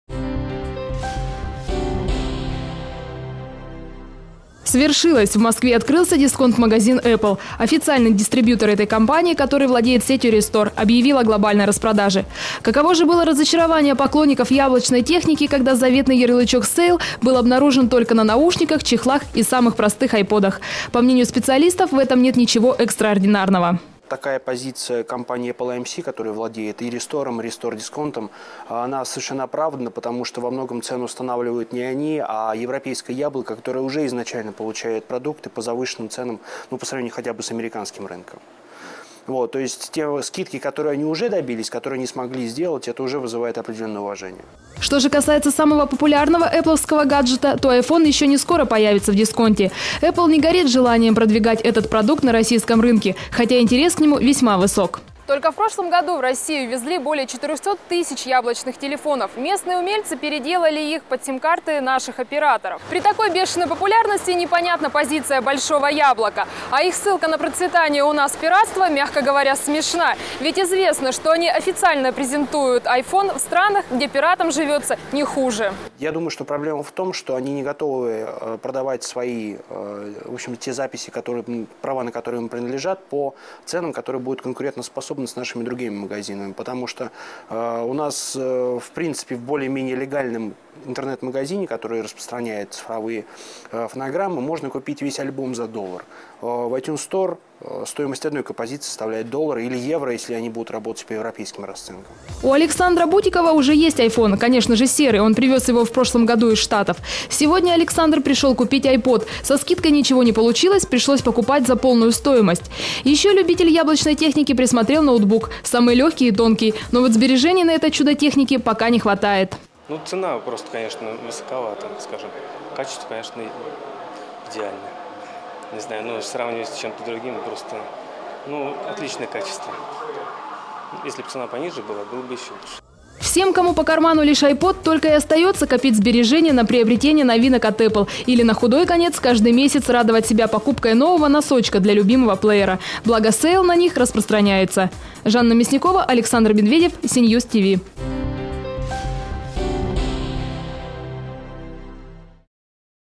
Apple открыл в Москве дисконт без скидок
О «сейле» было объявлено заранее, правда, потенциальные покупатели, мягко говоря, были разочарованы. Ярлычки «sale»  висели только на наушниках, iPod Socks и самых простых моделях айподов. Цены на остальной ассортимент ничуть не отличались от цен в других магазинах, официально реализующих «яблочную» продукцию. Многие надеялись встретить там iPhone, ставший уже легендой.